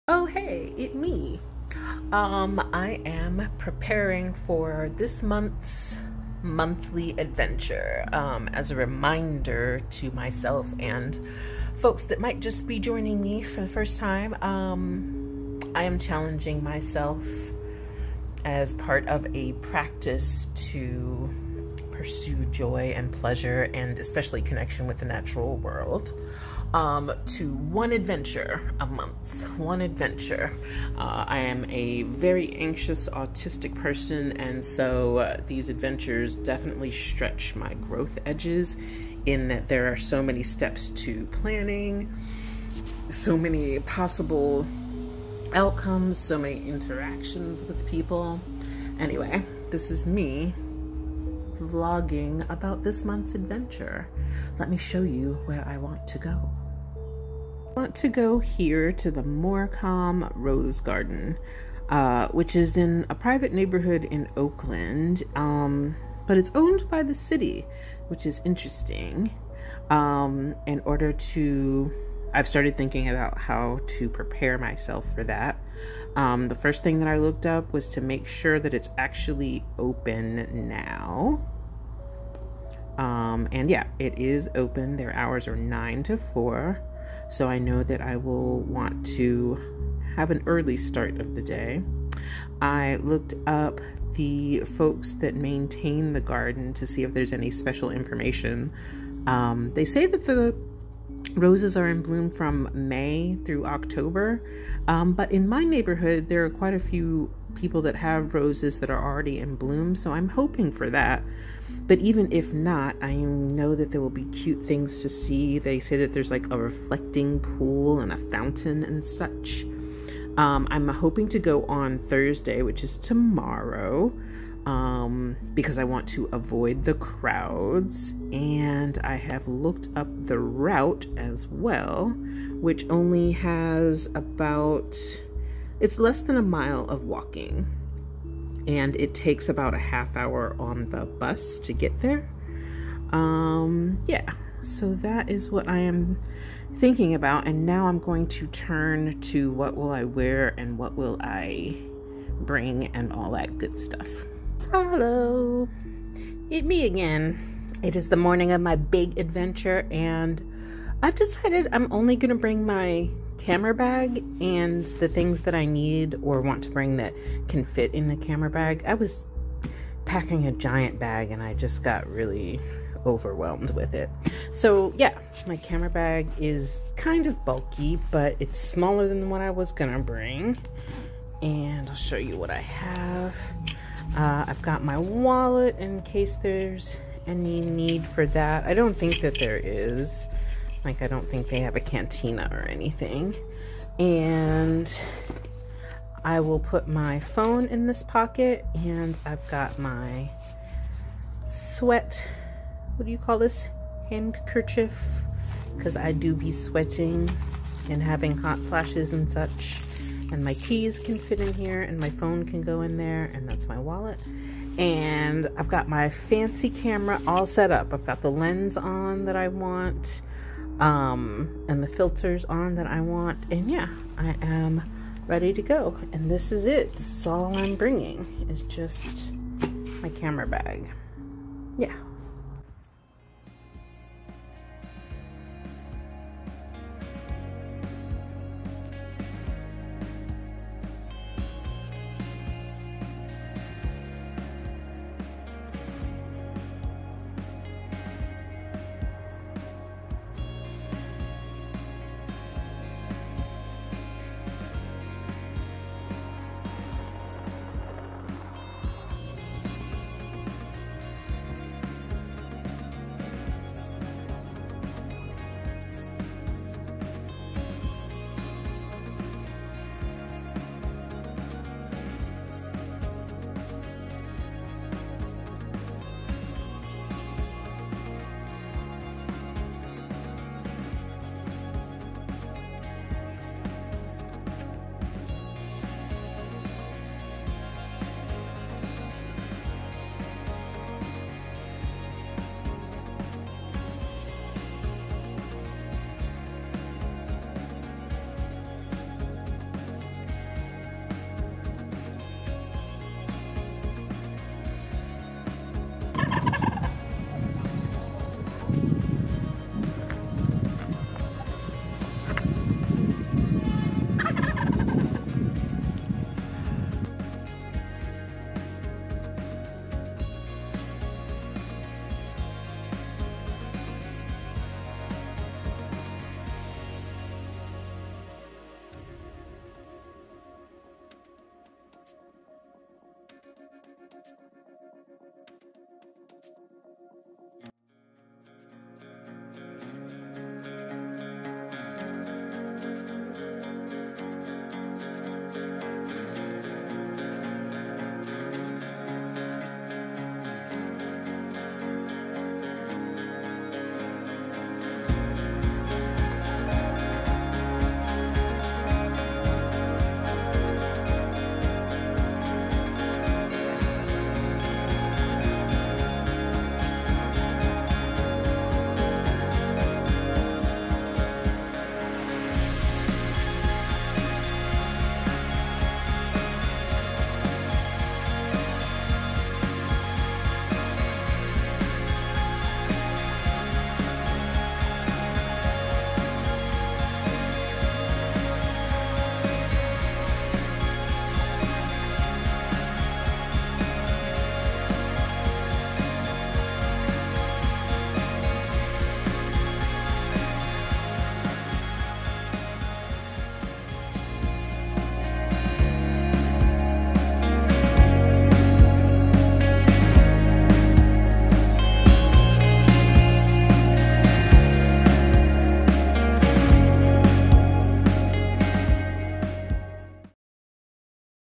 Adventures of an Anxious Autist Vol. 2 Morcom Rose Garden
your anxious autistic friend and her monthly adventures. this month we go to Oakland's gorgeous, accessible rose garden. Morton Rose Garden can be found at 700 Jean St, Oakland. is open from 9a-4pm daily and is in bloom through October.